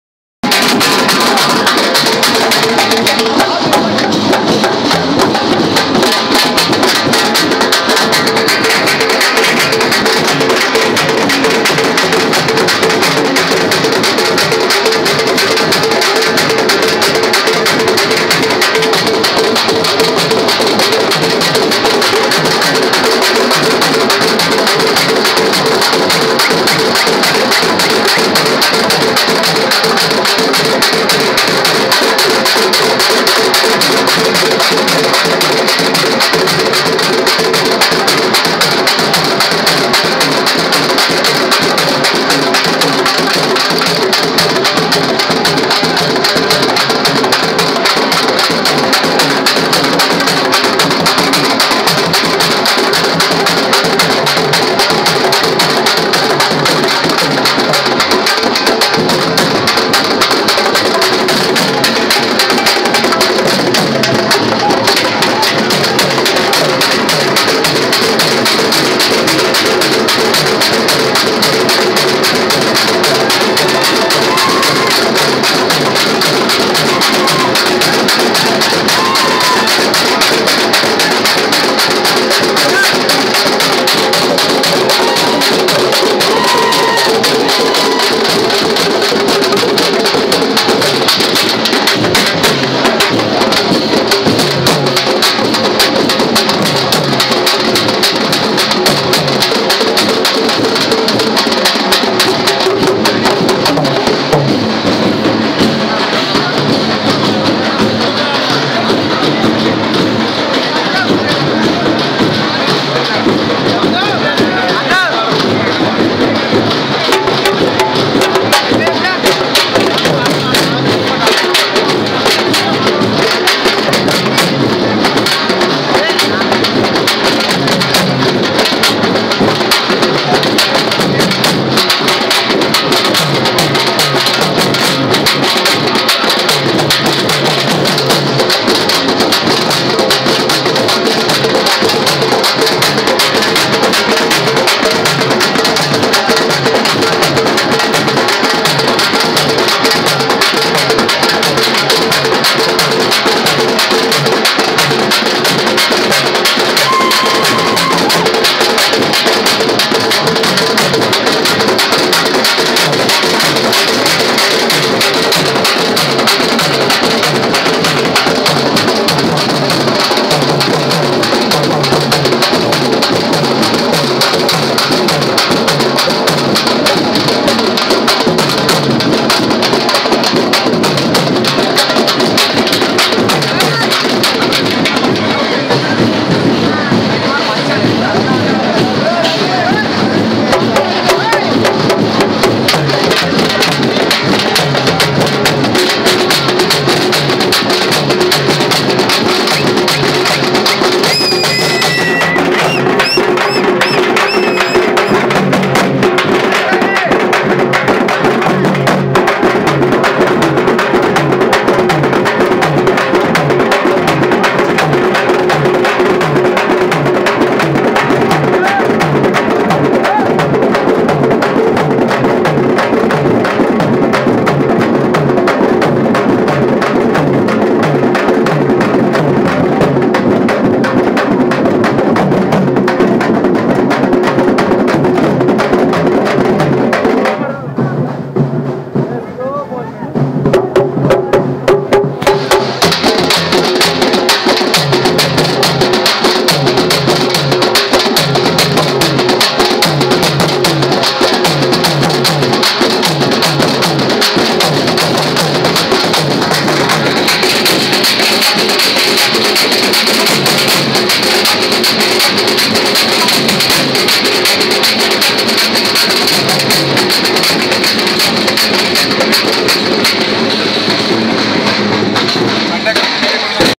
Ganesh celebration 2014 part 6
Sree siddivinayaka friends youth association( bmk and his friends dance in lalapet)